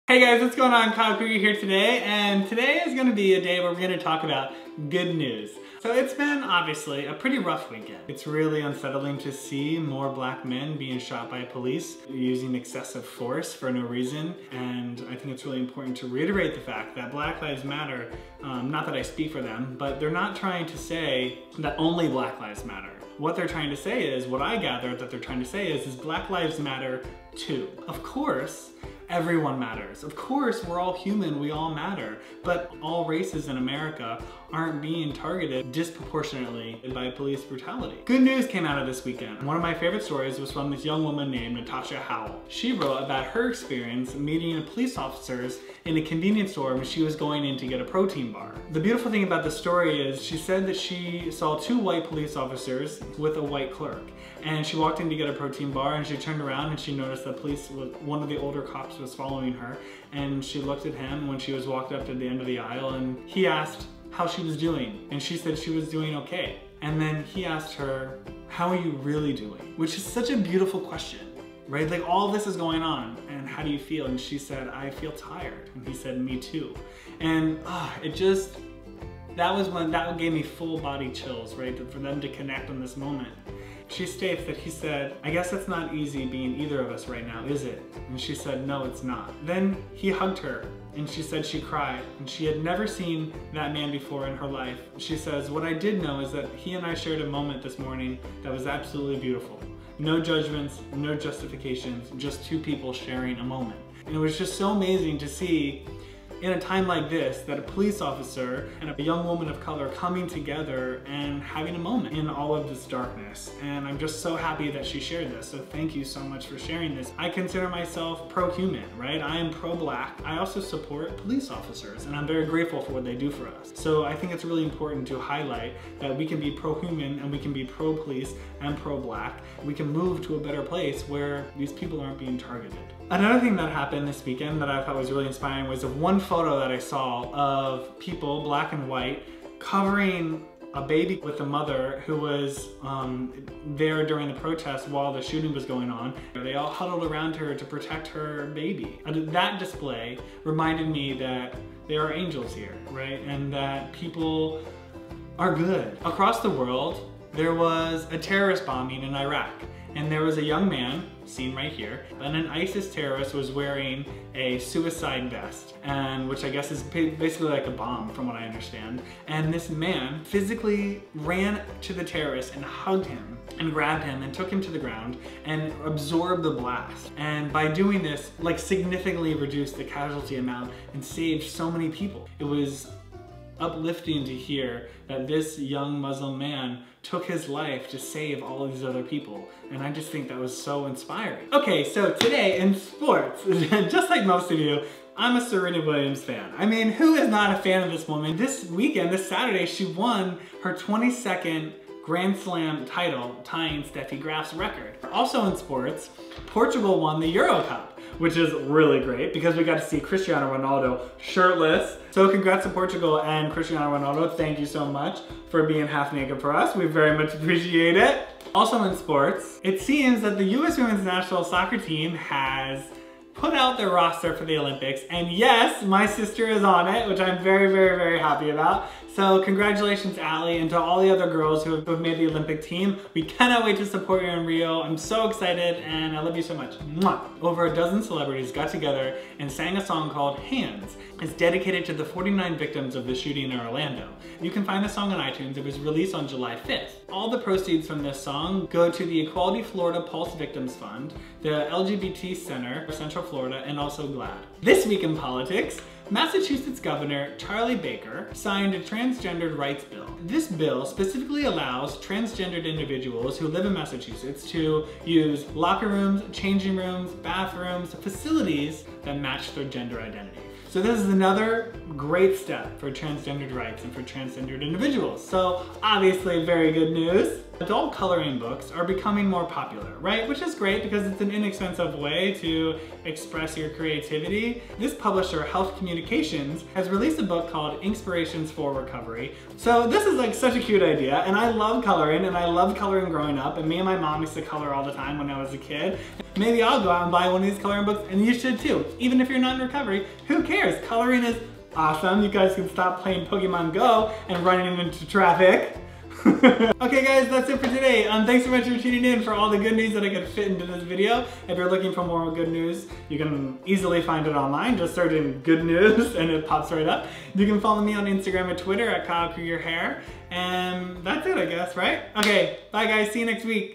I GOT SOME GOOD NEWS FOR YA!
There was some rough, sad news this past week.. but within all the darkness there was lots of light! I made a video highlighting all the good news I could fit into 5 minutes.

FIND KYLE:

Snapchat  kylekriegerhair